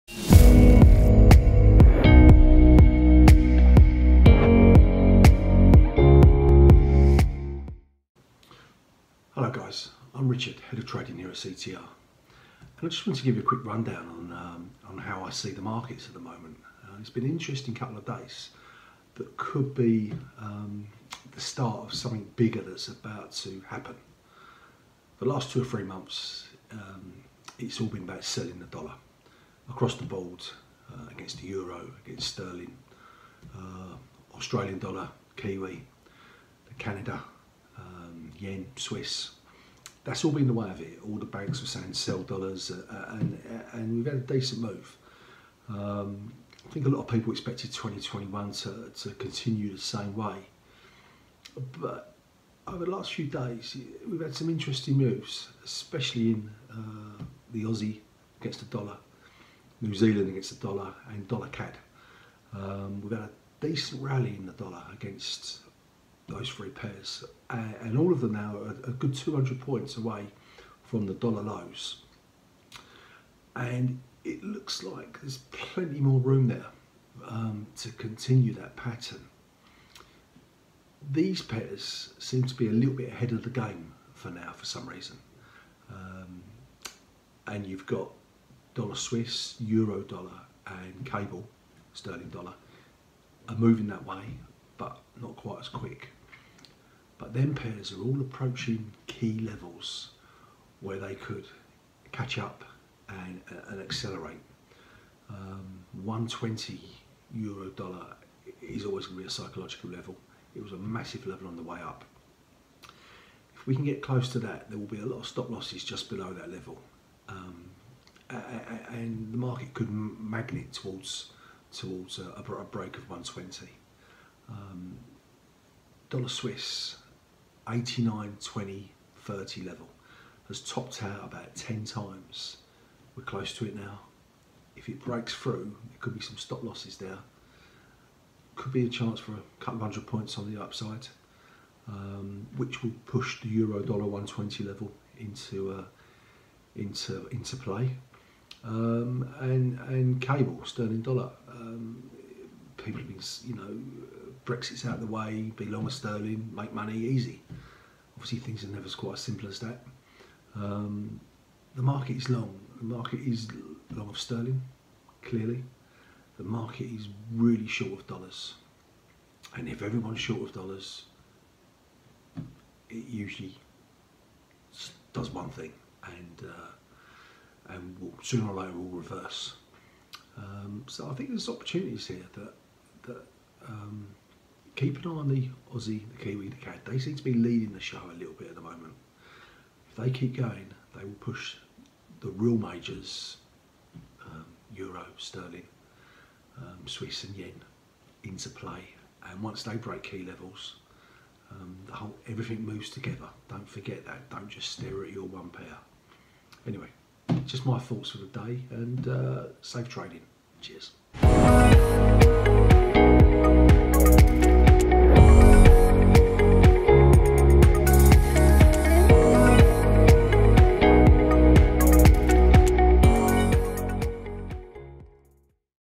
Market Update with Head of Trading, Richard Campbell 📆
In this video CTR's Head of Trading, Richard Campbell gives us a quick run down on the current market conditions and what moves he expects to see over the next few weeks.